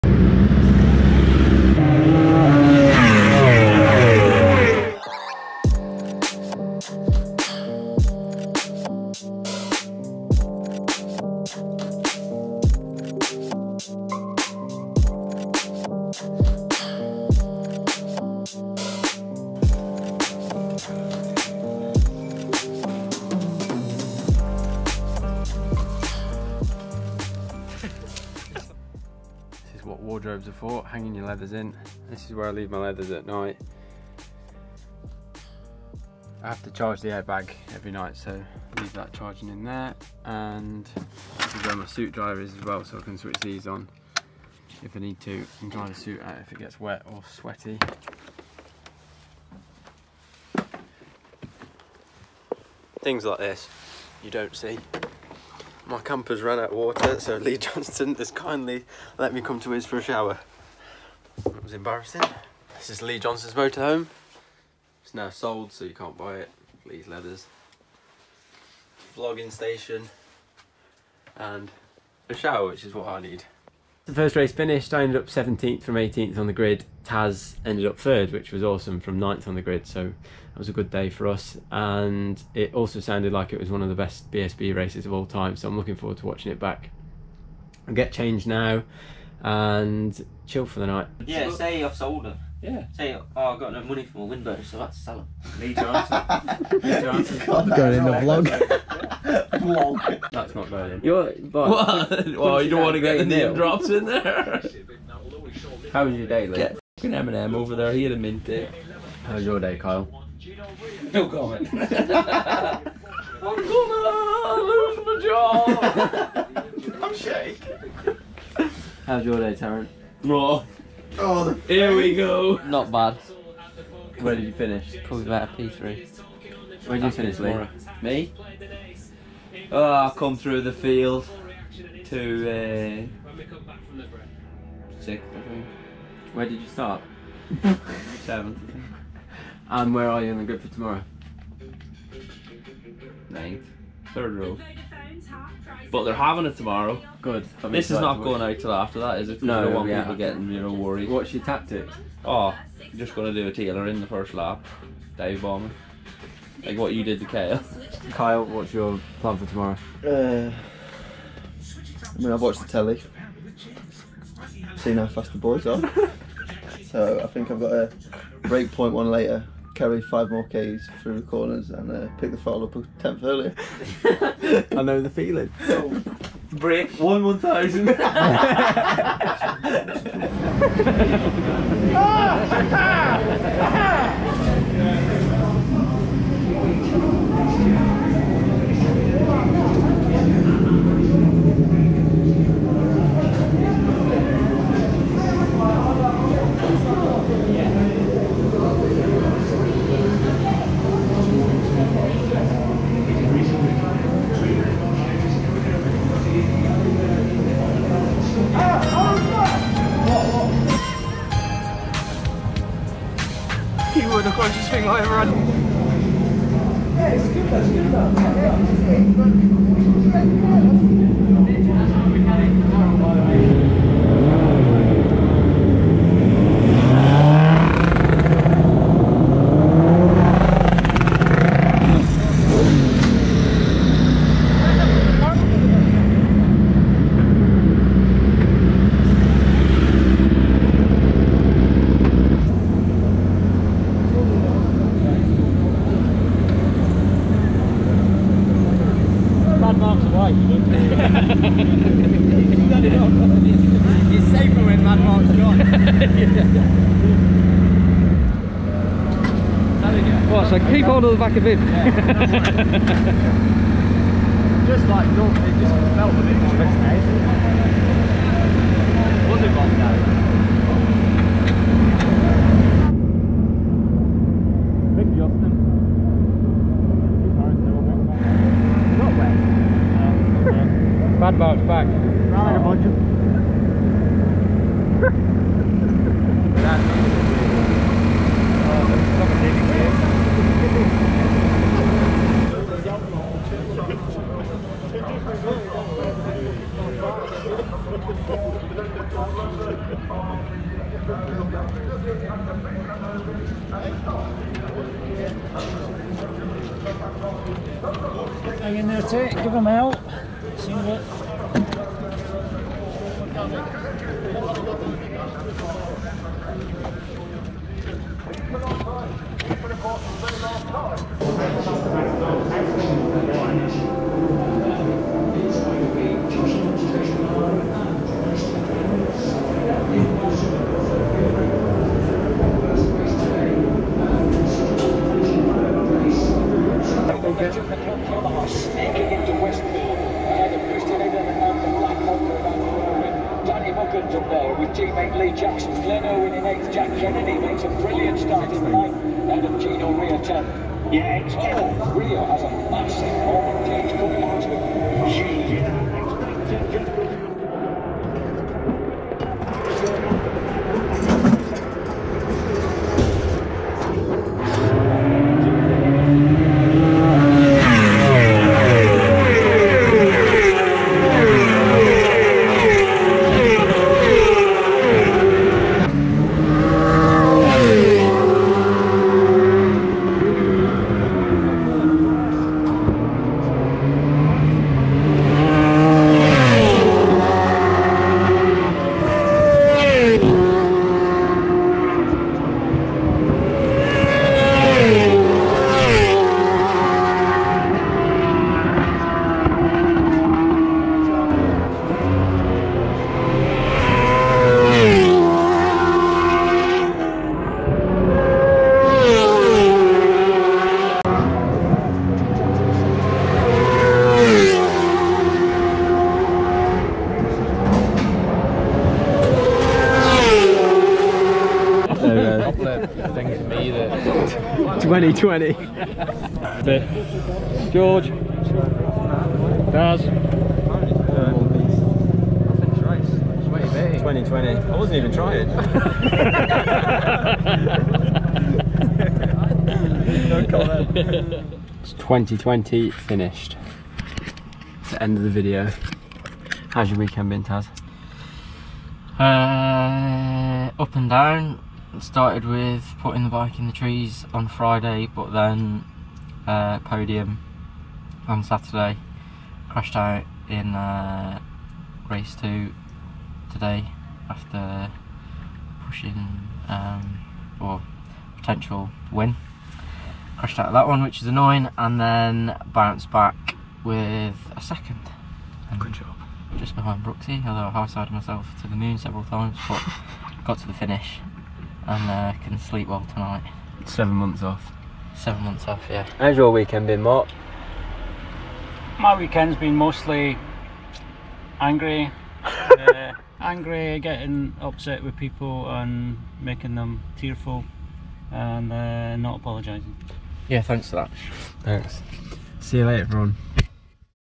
A day in the life of a motorbike rider - Brands Hatch British Superbikes 2020
I managed to take my camera to Brands Hatch to show you what a day in the life of a motorbike racer at the British Superbike Championship looks like.

MY GEAR:
Shoei helmet
Spidi gloves
Sidi boots
Spidi leathers